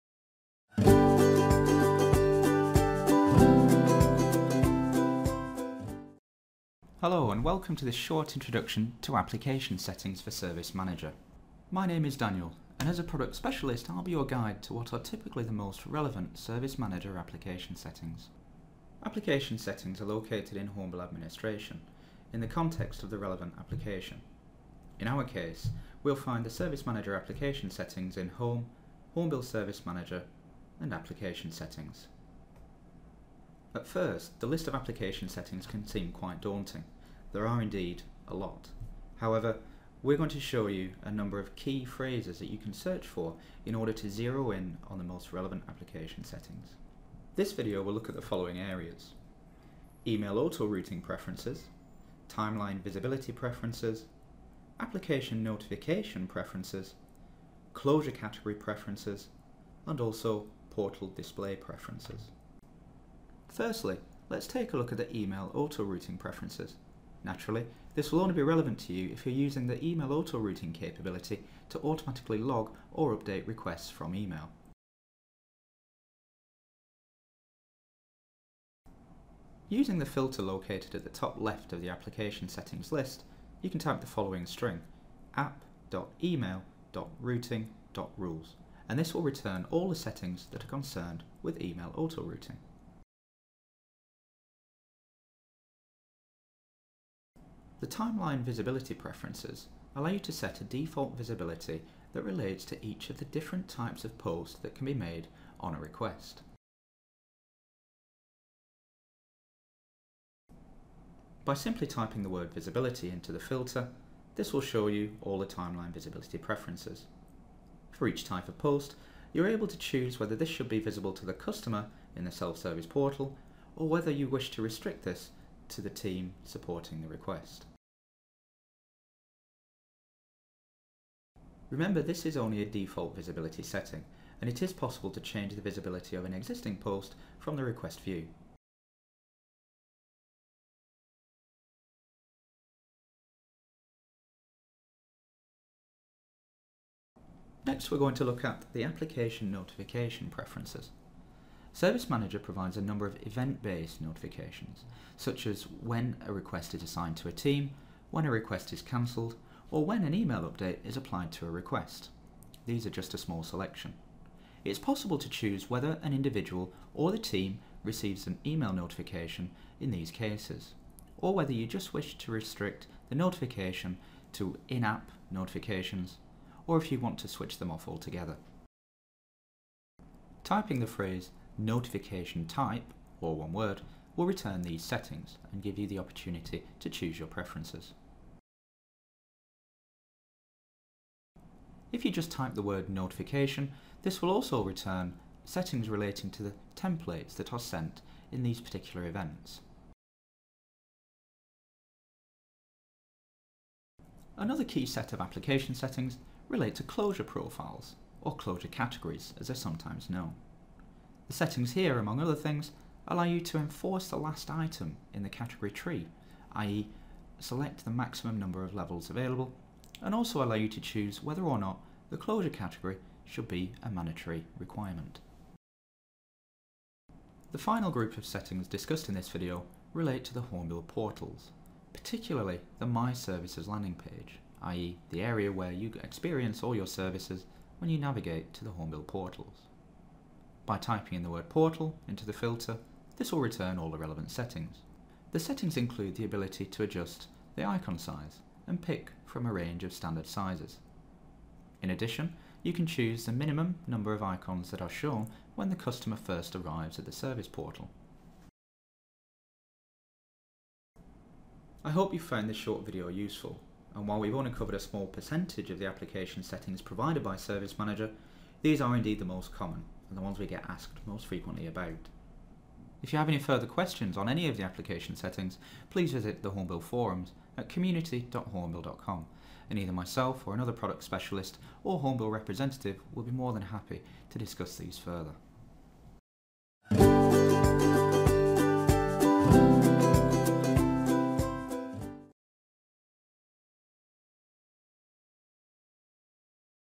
Introduction to Service Manager Application Settings
A brief overview to some of the available application settings that exist in the Service Manager application.